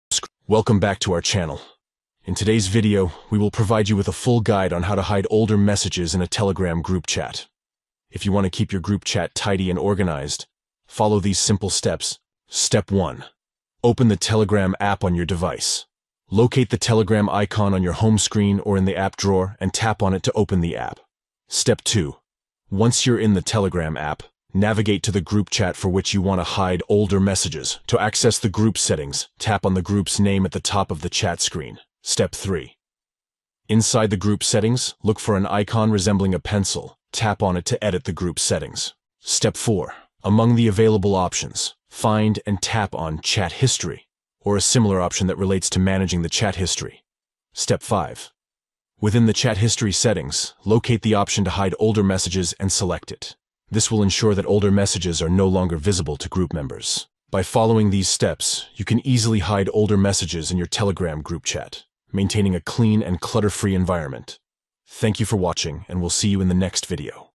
How To Hide Older Messages In Telegram Groupchat - Full Guide
Learn how to hide older messages in a Telegram group chat with our full guide. Follow these steps:

1. Open Telegram.
2. Tap on the group's name and tap on the pencil icon.
3. Tap on "Chat History" and tap on "Hidden".